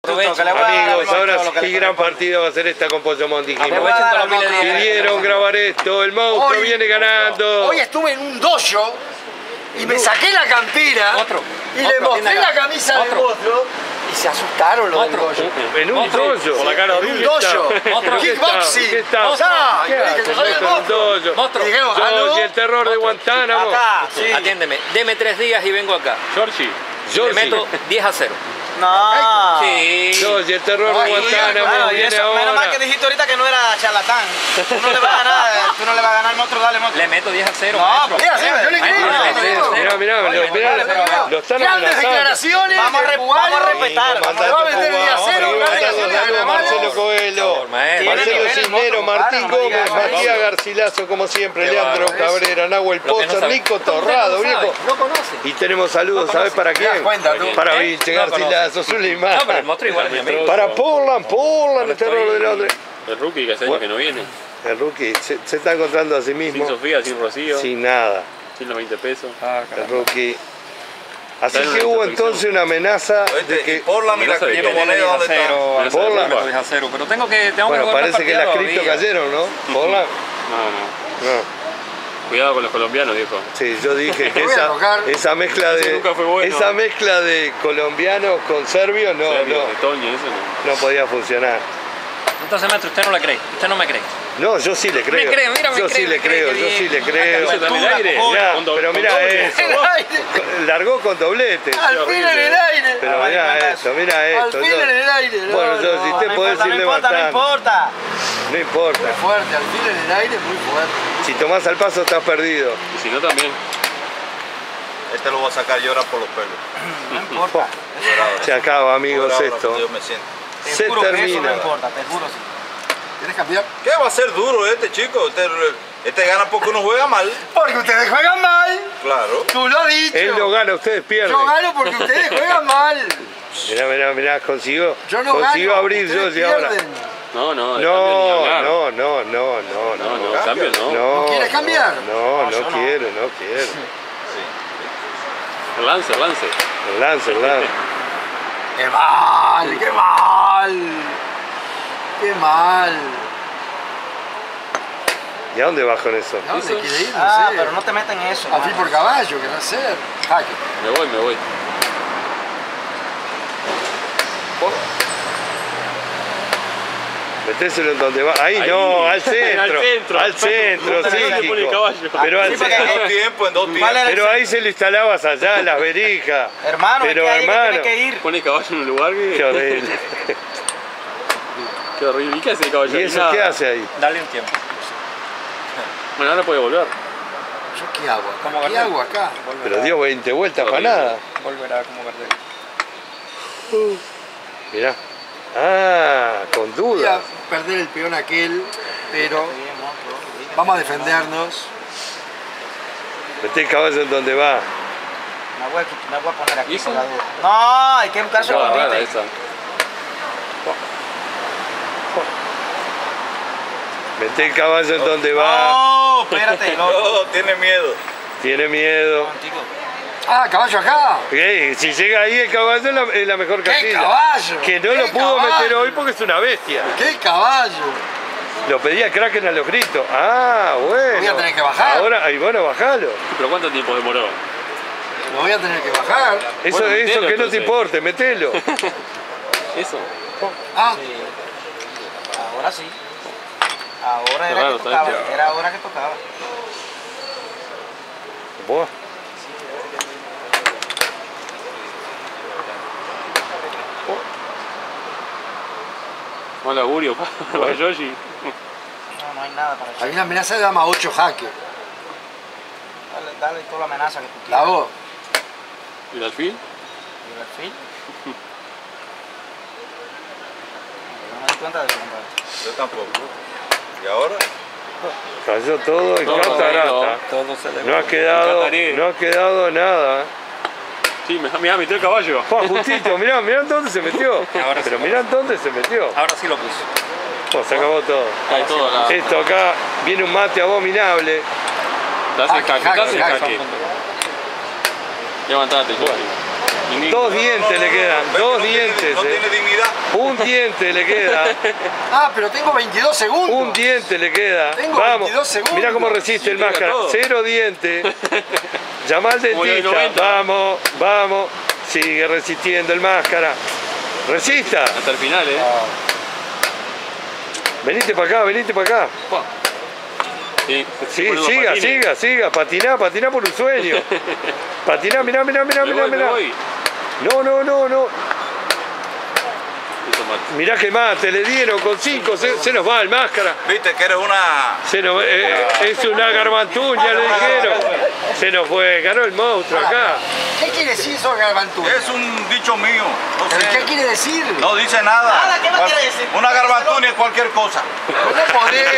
ajedrez callejero (la argolla con doblete)
NUEVO , Amigos que gran partida fue esta, el monstruo venía modelando las remeras egomaníacas y dando la comida, haciendo calentar a rolando y los demás cubanos, y aparece yoyi el máscara negra que viene a sacar la cara por todos y cae en una colgada con doblete con los caballos tremendos del monstruo que ya empieza a conversar, calladito el terro de guantánamo resiste lo que puede y busca con un burro que no instala en donde va aconsejado por el kraken de afuera, y empieza a caer todo en catarata, no le deja el monstruo comiendo hasta el último pollo y lo manda para afuera con un mate espantoso con 5 segundos en el reloj en una gran partida para sus fans, Bar Hispano, Montevideo,Uruguay.